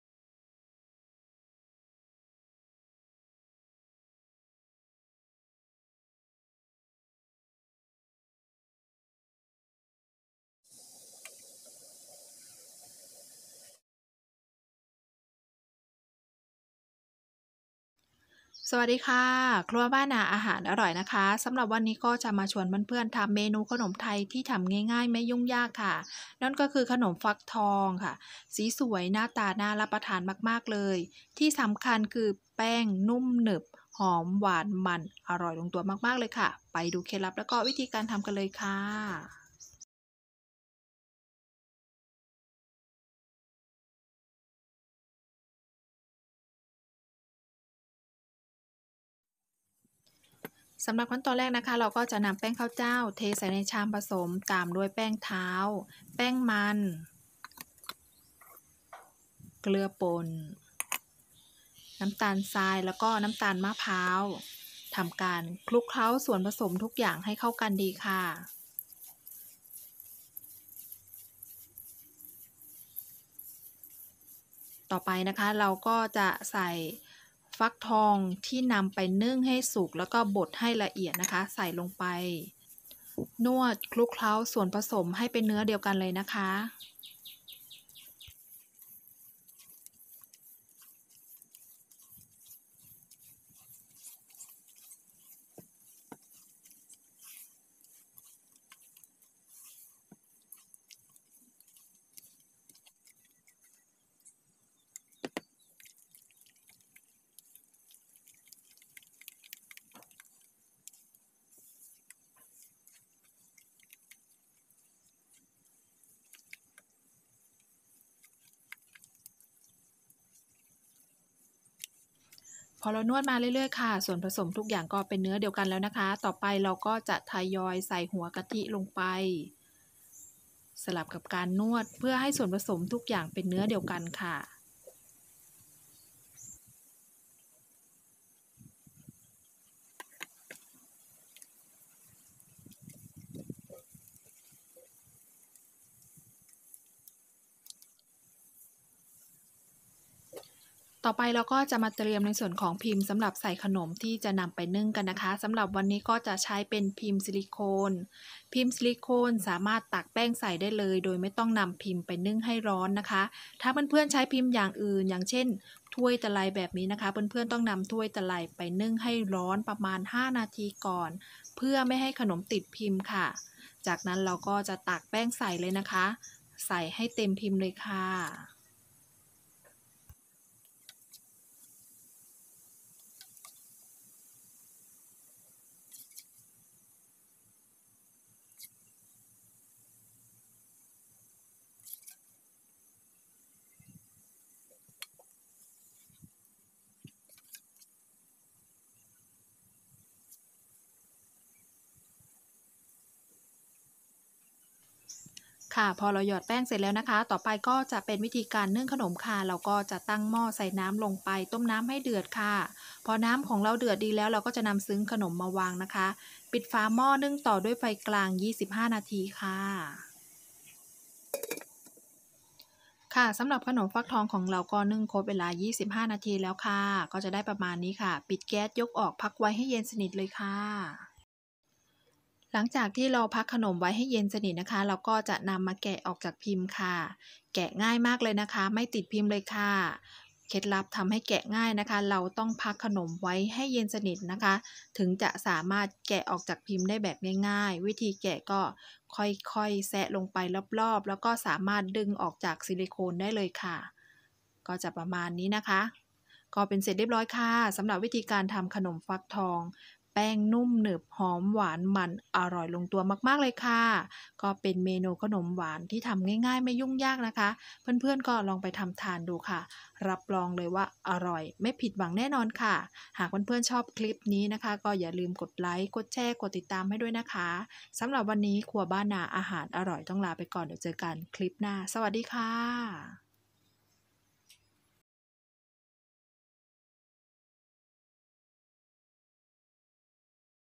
วิธีทำขนมฟักทองนุ่มหนึบหอมหวานอร่อยมากๆPumpkin Snacks
ขนมฟักทอง ขนมไทย Pumpkin Snacks
ฟักทองนึ่งบด160กรัม
กระทิ150ml
แป้งข้าวจ้าว40กรัม
แป้งท้าว40กรัม
แป้งมัน30กรัม
น้ำตาลทราย40กรัม
น้ำตาลมะพร้าว60กรัม
พิมพ์ซิลิโคน